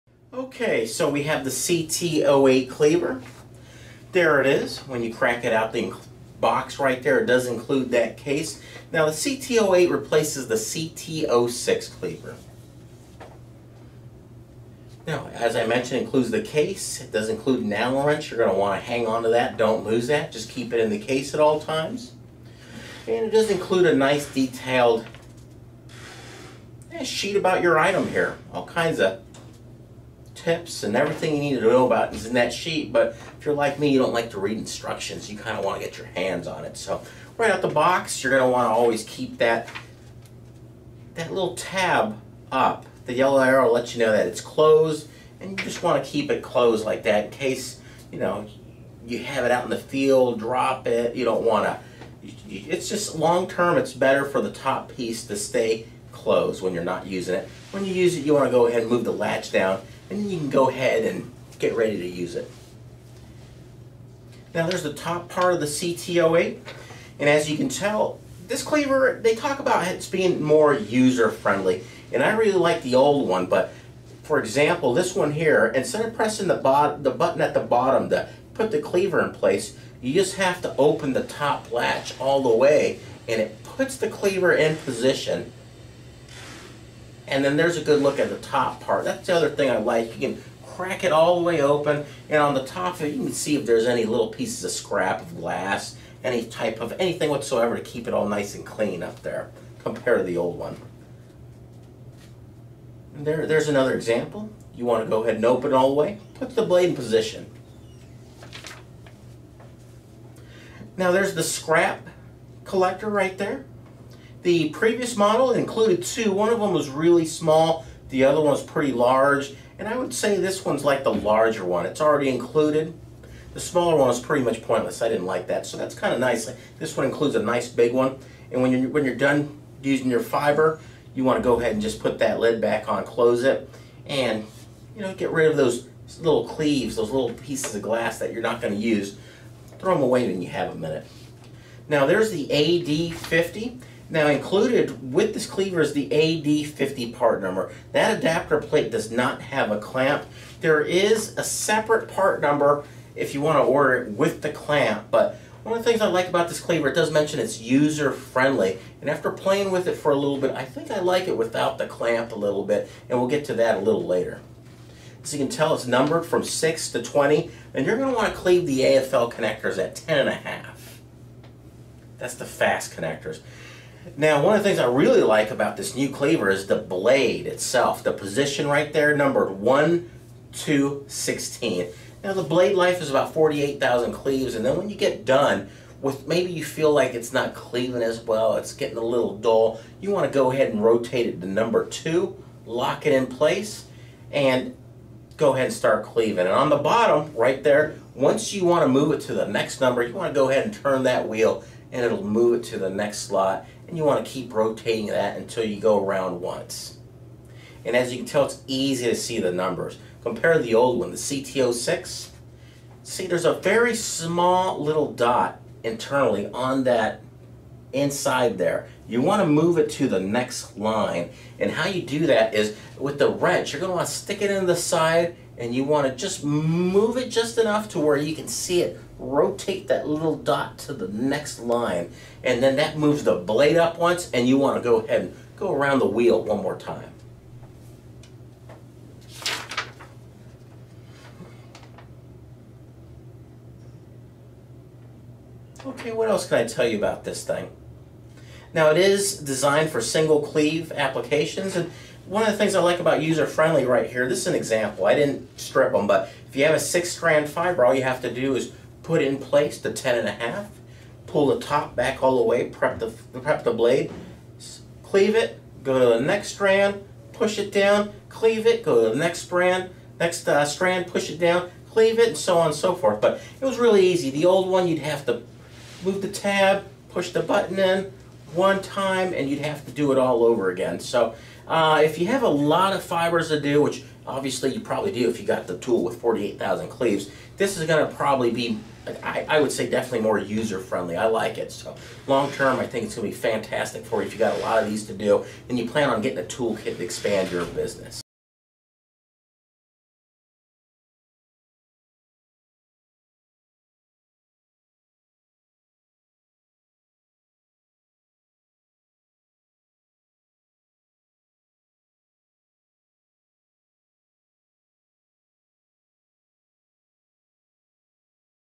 Fujikura CT08 - обзор возможностей автоматического скалывателя оптических волокон
В этом обзорном видеосюжете продемонстрированы основные достоинства недорогого автоматического скалывателя оптических волокон Fujikura CT08, а именно: отличная точность и стабильность скола, возможностью поворачивать нож без использования дополнительных инструментов, простая процедура замены ножа и защищённость скалывателя от пыли, влаги, ударов и падений.

Полный перечень сварочных аппаратов и скалывателей для оптоволокна представлен на странице